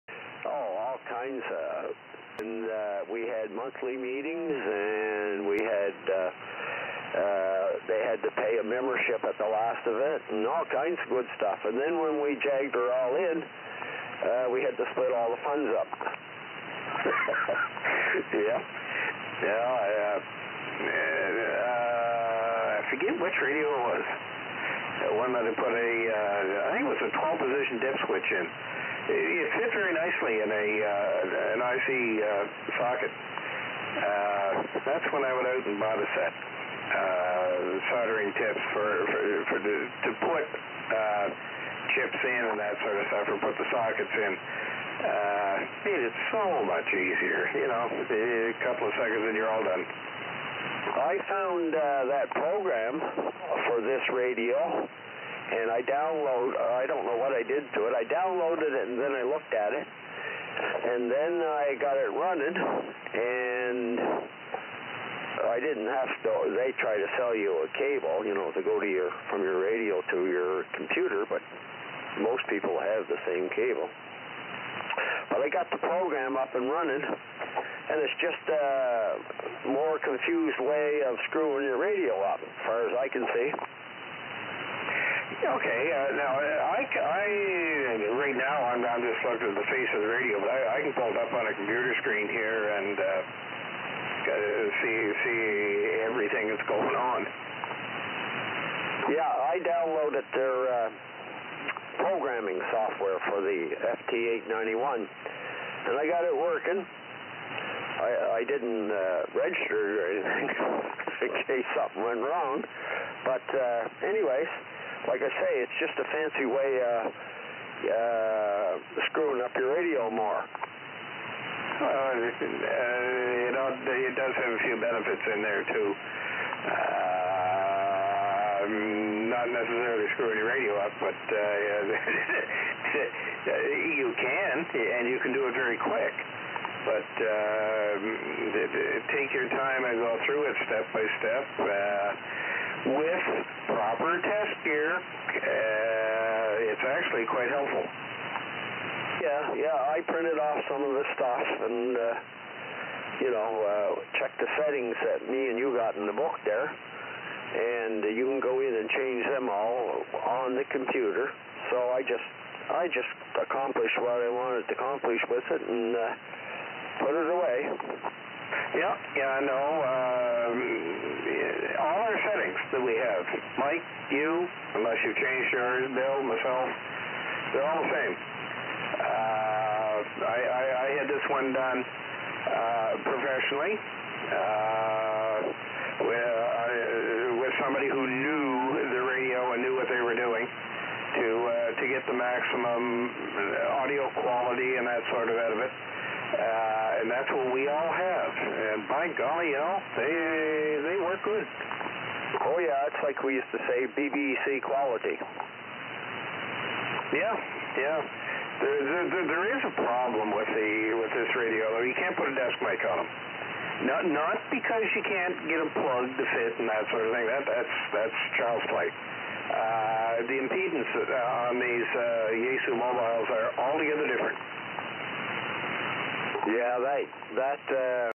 Local CB Action in LSB 30/04/2020 23:28 UTC
This is the first time i have ever heard any local CB traffic here in Nova Scotia.

Heard on the SDRPLAY RSP1A and Wellbrook Loop.